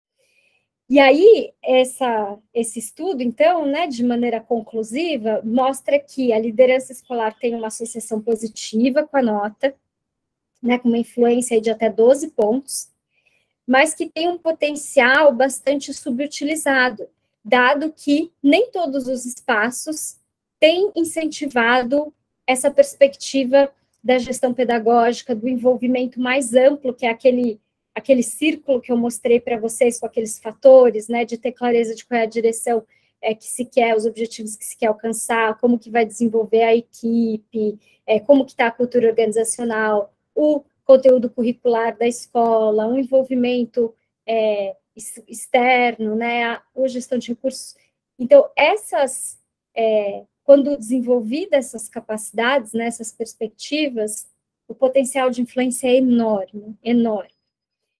Pesquisadora Filomena Siqueira fala sobre escolaseficazes em seminário da Cátedra SHF
Trecho do Seminário 2, do Ciclo de Seminários A BNC das Competências Gerais do Diretor Escolar, ministrada pela pós-doutoranda da Cátedra Sérgio Henrique Ferreira Filomena Siqueira, trazendo alguns resultados da pesquisa qualitativa.